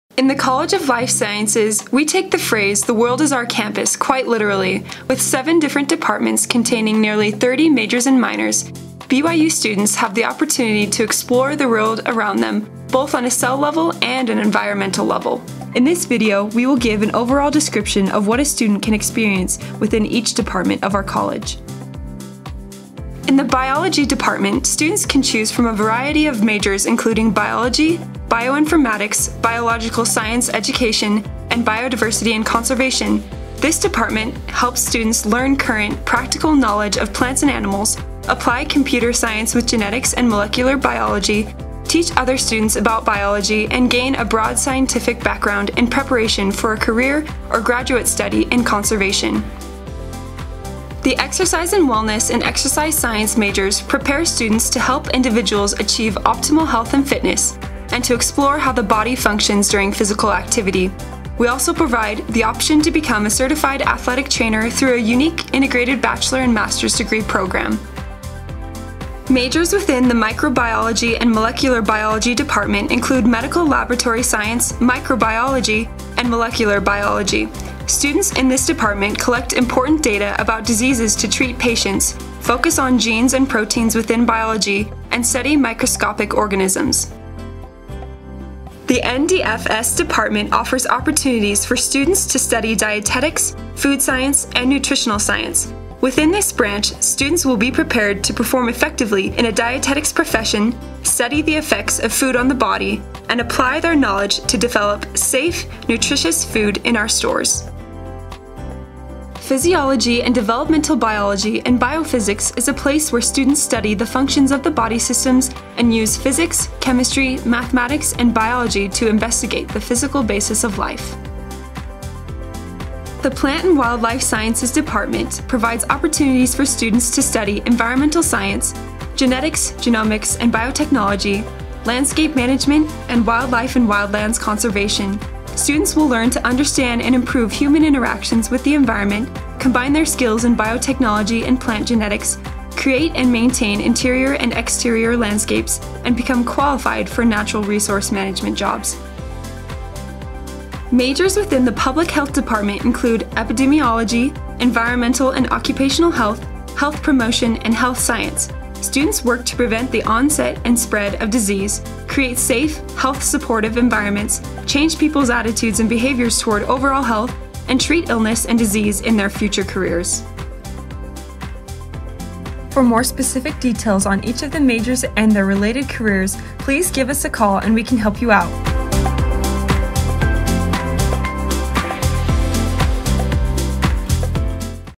BYU College of Life Sciences Introduction
Video about all the majors in the College of Life Sciences